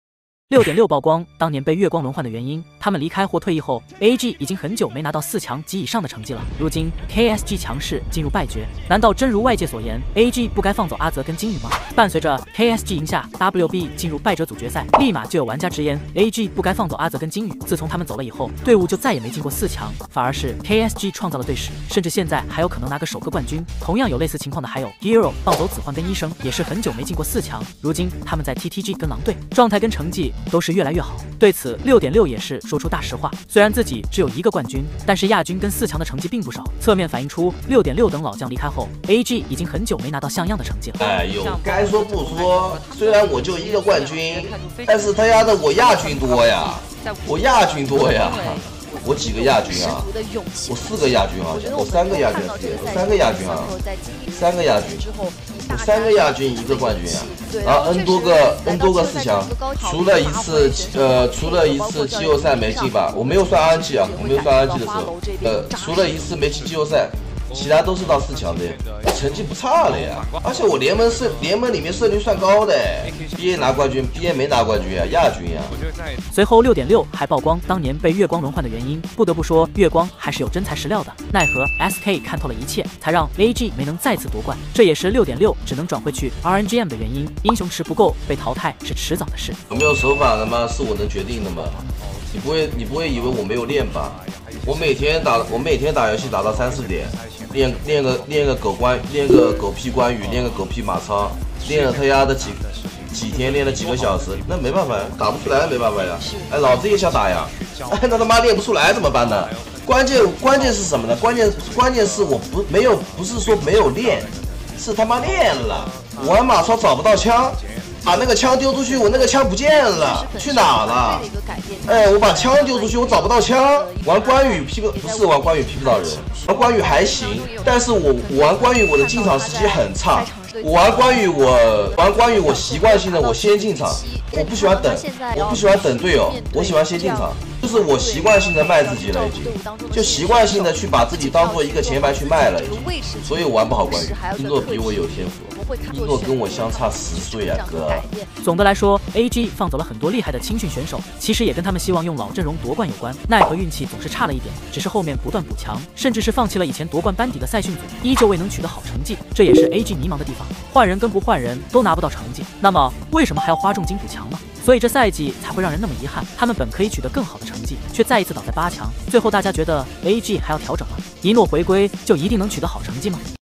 爆！六点六曝光被轮换的原因，老将离开不进反退，成都AG超玩会跟Hero成难兄难弟 | 王者 | 王者荣耀 | 2023KPL夏季赛 季后赛
发布王者荣耀KPL原神视频
感谢大家支持
谢谢大家
如果大家有什么想看的也可以留言告诉我